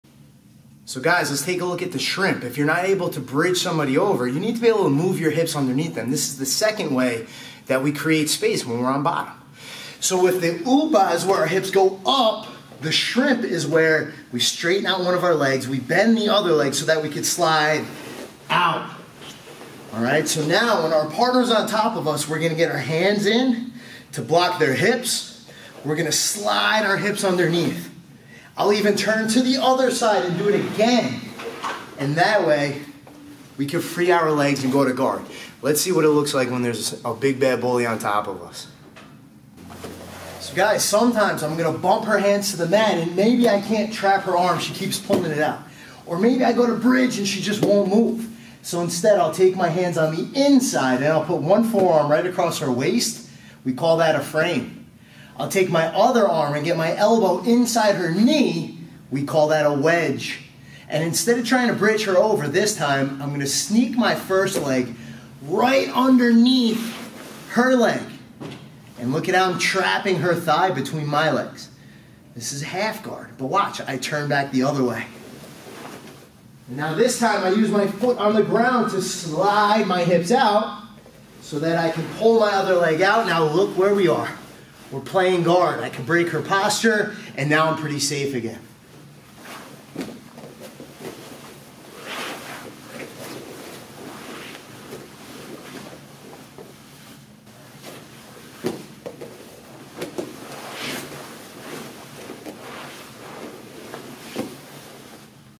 Stuck on Bottom? "Shrimp" Back to GUARD! | the Hip Escape
You don't have to be a black belt to know that you don't wanna be stuck on bottom in a fight.

But sometimes, bringing someone over who's much bigger, stronger, or even more skilled than you is easier said than done.

Luckily, Jiu Jitsu always gives us a way out!  Here, we use the shrimp- also called the hip escape- to move out from underneath our opponent and get back to a BETTER position: the GUARD!